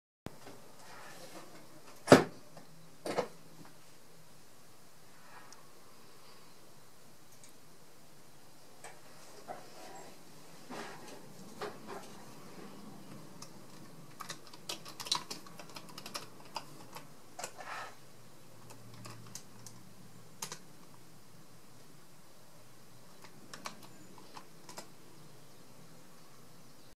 Mandelbrot in BBC Basic (RISCOS, Raspberry Pi)
Fractal plotted in mode 1 - colour dithering is by the OS. Can run up to 1920x1080 with 32bit colour but that would be slower and less retro.